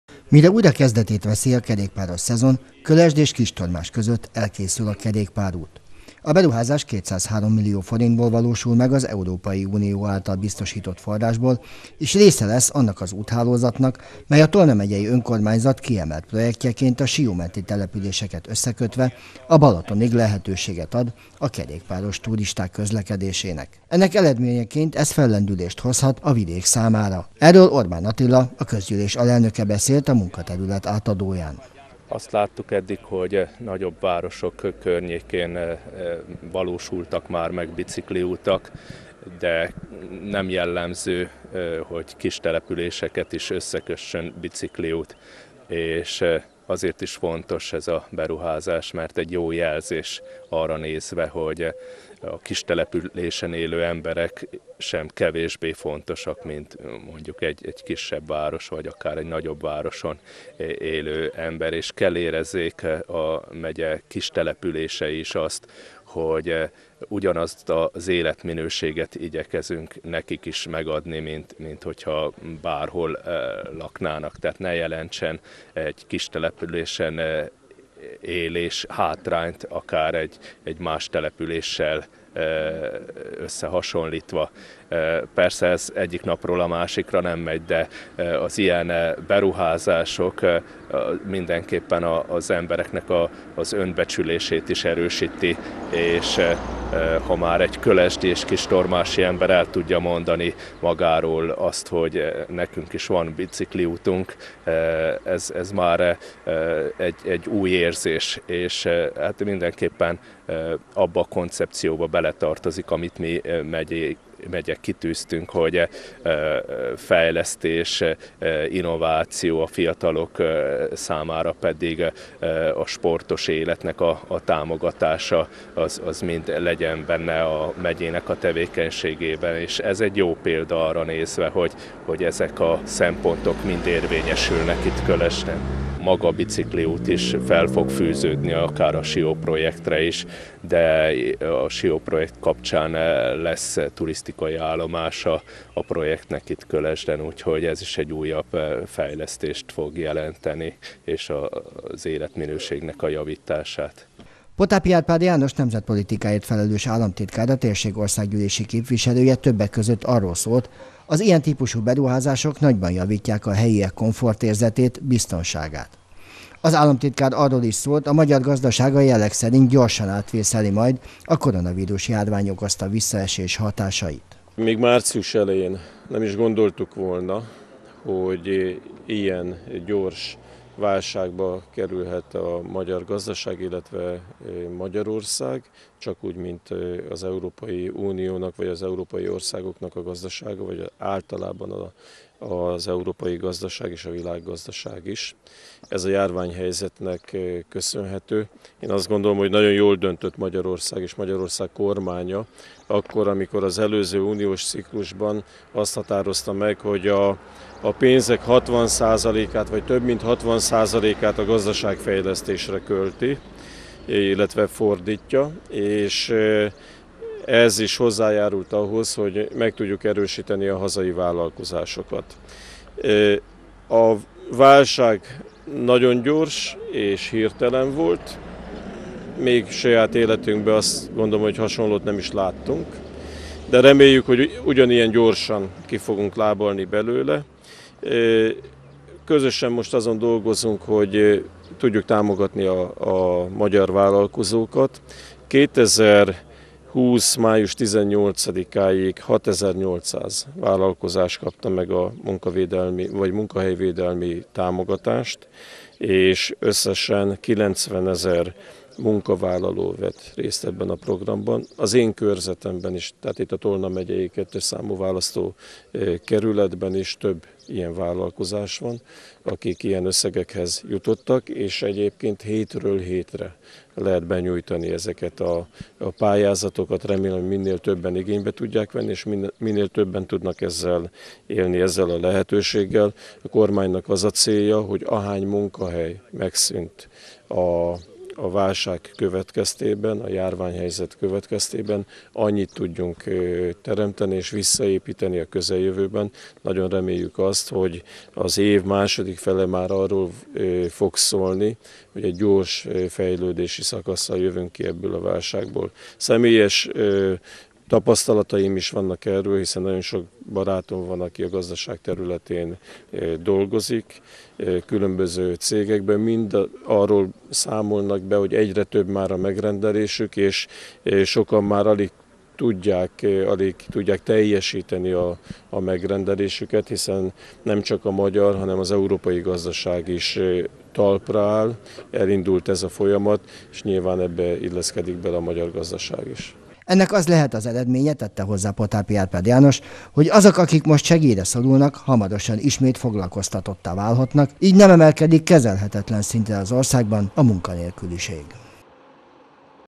Átadták a Kölesd és Kistormás között épülő kerékpárút munkaterületét a kivitelezőknek. 20200521
Átadták a Kölesd és Kistormás között épülő kerékpárút munkaterületét a kivitelezőknek. Potápi Árpád János államtitkár, a térség országgyűlési képviselője azt mondta, az ilyen típusú beruházások egyszerre szolgálják a helyben élők komfortérzetének erősödését és kapcsolhatják be a területet az idegenforgalom vérkeringésébe. Arról is szólt, hogy az ország véleménye szerint jó úton van afelé, hogy gyorsan kilábaljon a koronavírus járvány okozta gazdasági válságból.